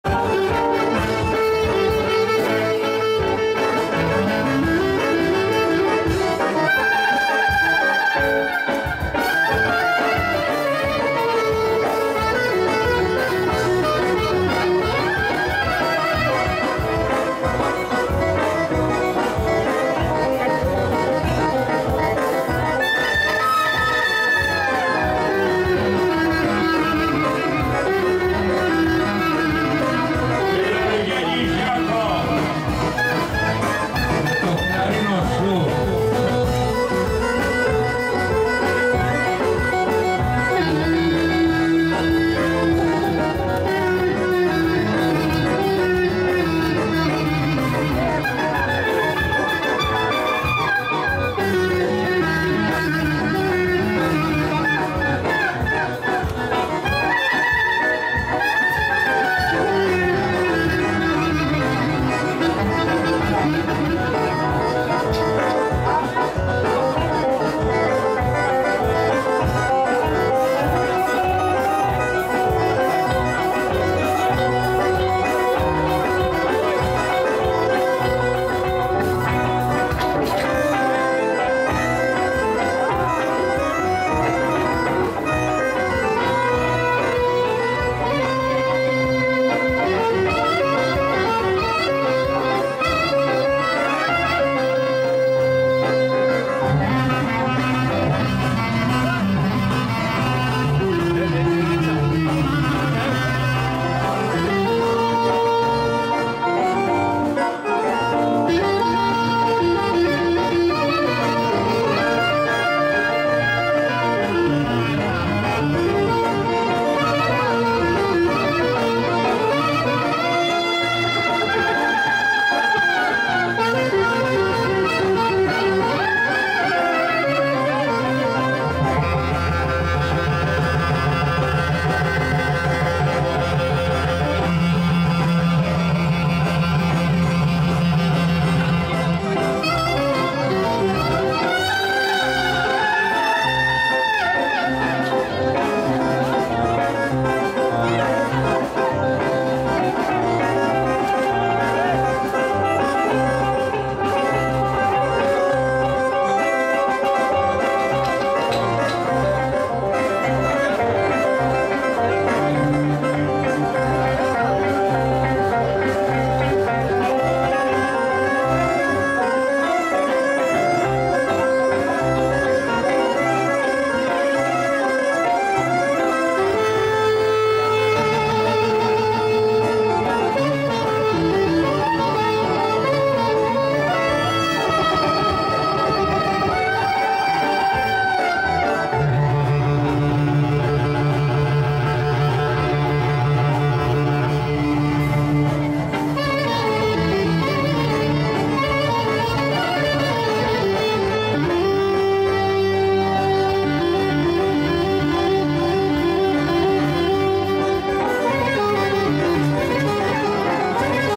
ΖΙΑΚΟΣ ΓΙΑΝΝΗΣ ΣΠΑΕΙ ΤΑ ΚΛΑΡΙΝΑ ΣΤΟ ΠΑΝΗΓΥΡΙ ΤΗΣ ΣΥΚΕΑΣ ΚΑΙ ΑΝΑΤΙΝΑΖΕΤΑΙ ΤΟ ΜΑΓΑΖΙ, ΣΟΛΟ ΚΛΑΡΙΝΟ
ΣΤΟ ΠΑΝΗΓΥΡΙ ΤΗΣ ΣΥΚΙΑΣ ΣΤΗΝ ΨΗΣΤΑΡΙΑ Ο ΤΑΚΗΣ